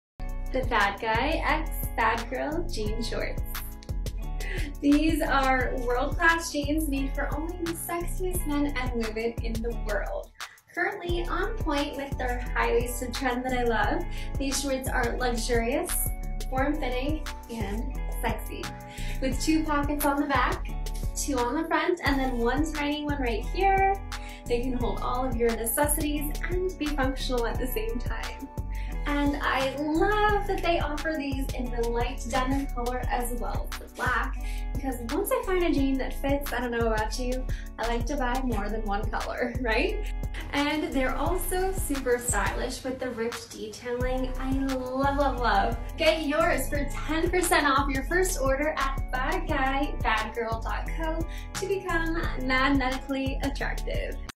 These Shorts 😍😍😍
Here's a review of our lovely BADGUY x BADGIRL Denim Shorts. She really likes it, and so will you...